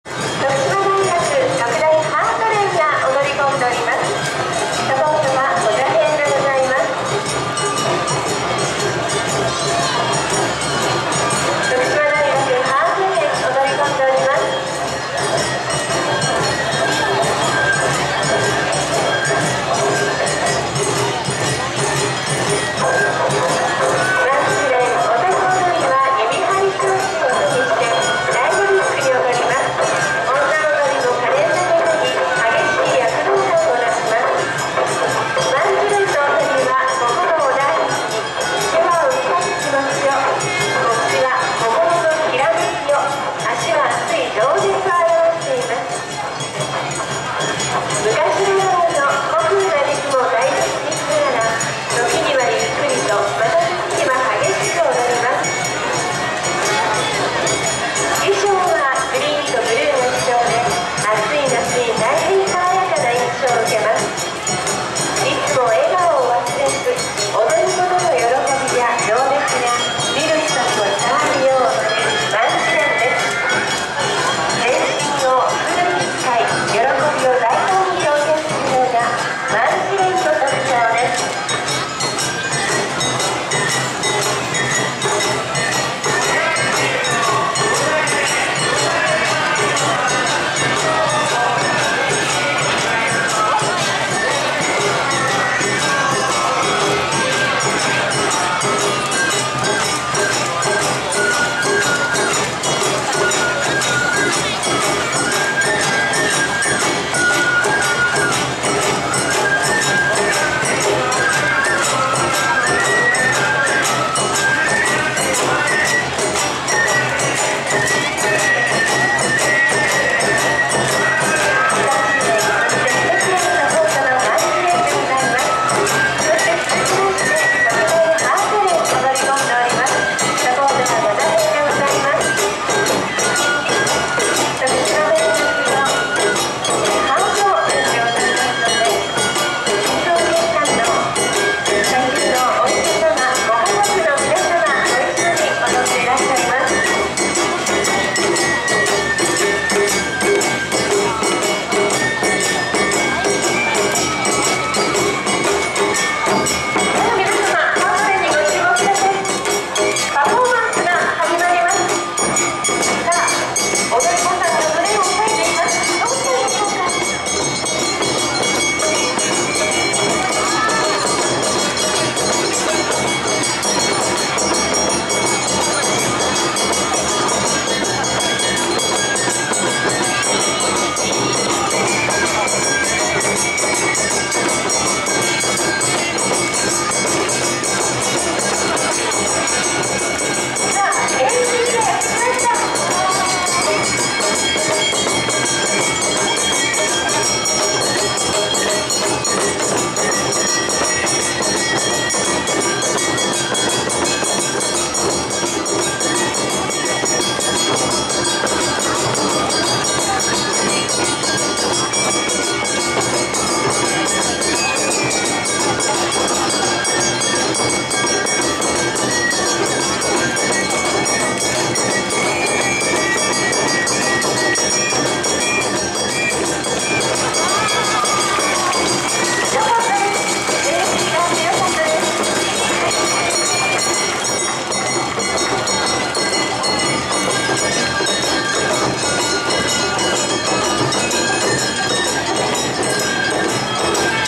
ハート連（徳島大学病院）／紺屋町演舞場／徳島市阿波おどり2016
2016/08/15
徳島市阿波おどり2016（四日目）
【会場】徳島市内各所の有料演舞場、無料演舞場、おどり広場ほか
【主催】徳島市観光協会、徳島新聞社
【主管】阿波踊り実行委員会
【後援】徳島県、徳島市、徳島商工会議所